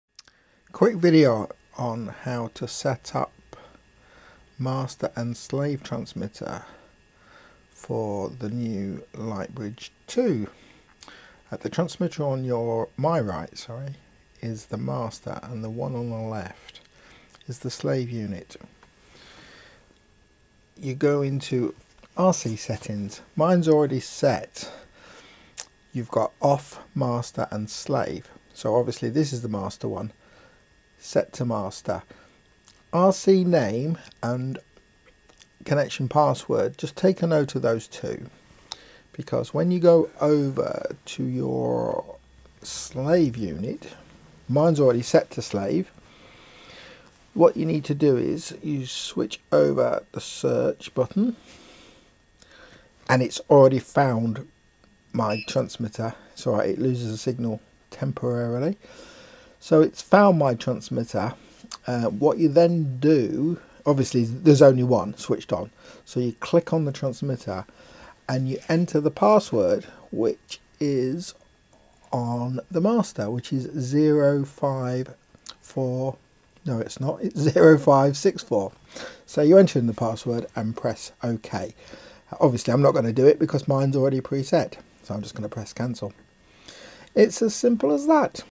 DJI Lightbridge 2 Ground Unit
Setting up DJI Lightbridge 2 Ground Unit master and slave.

Slave ground Unit give you control over the gimbal.